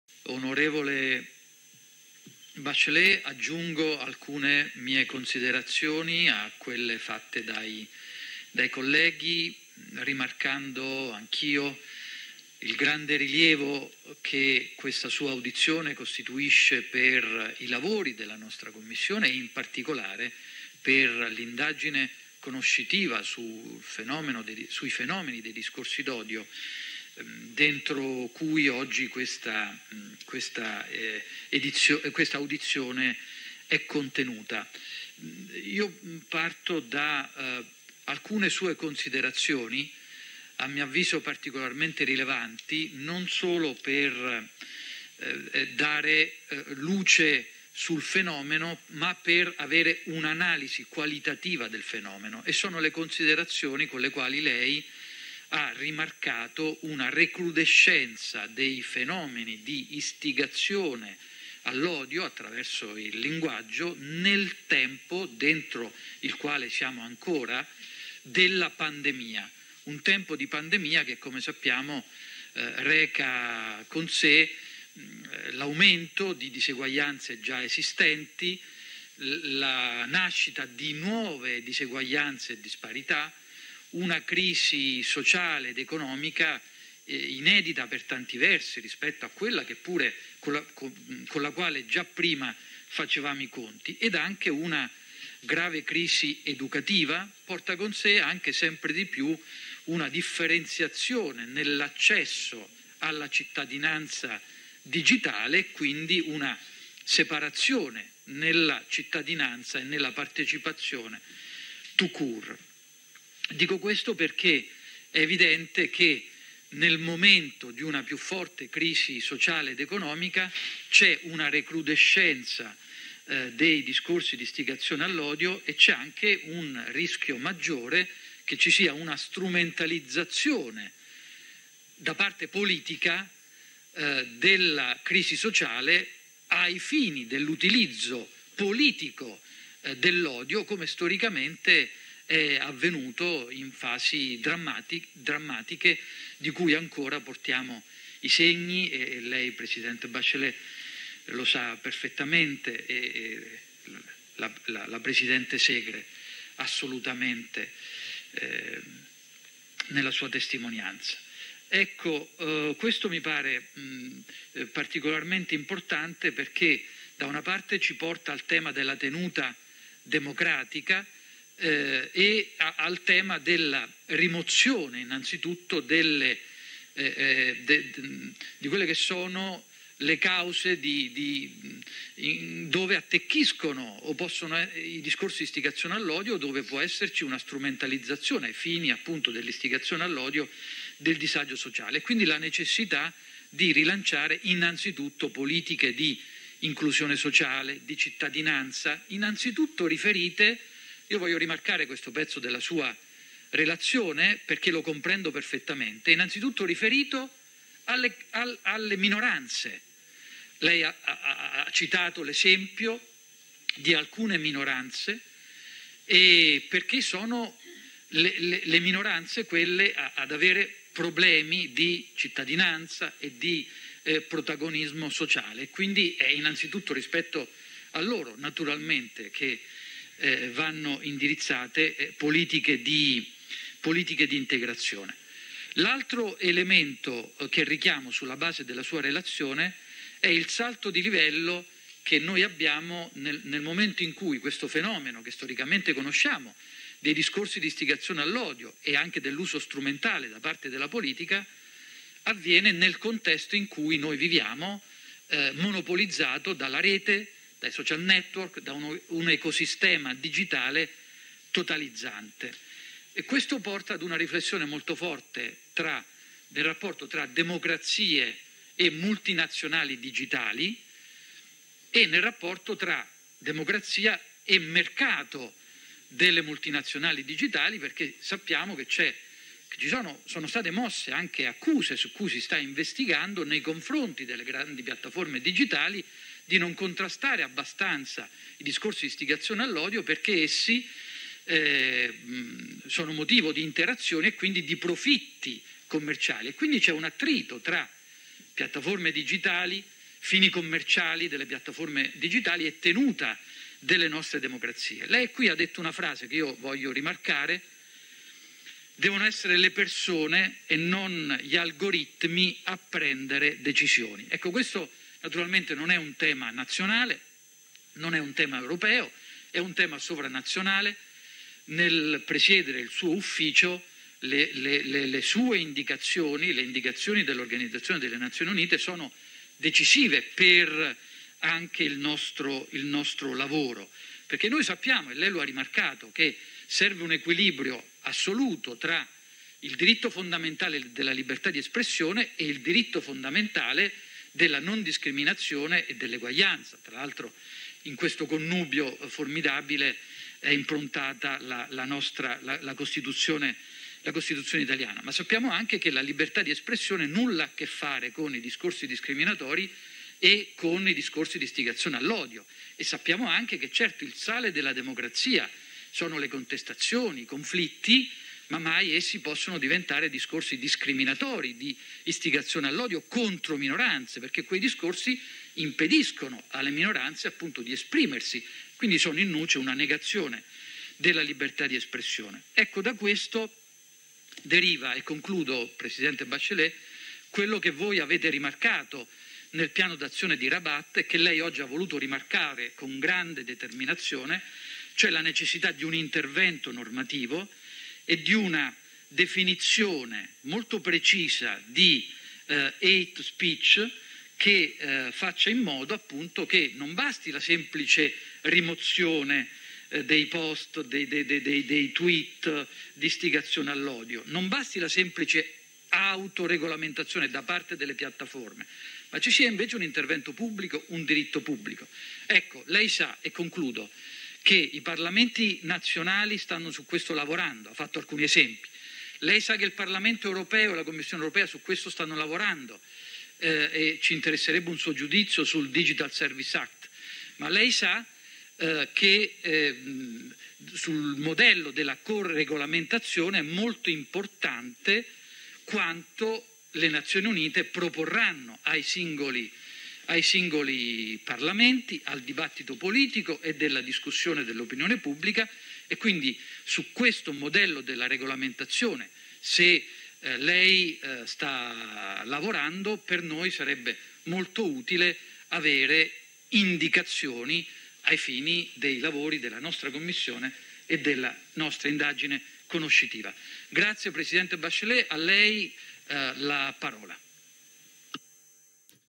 Intervento di Francesco Verducci a margine dell'audizione di Michelle Bachelet in Commissione Segre
Intervento di Francesco Verducci a margine dell'audizione dell'Alto Commissario delle Nazioni Unite per i diritti umani, Michelle Bachelet, in Commissione straordinaria intolleranza, razzismo, antisemitismo, istigazione all'odio e alla violenza

20 Gennaio 2022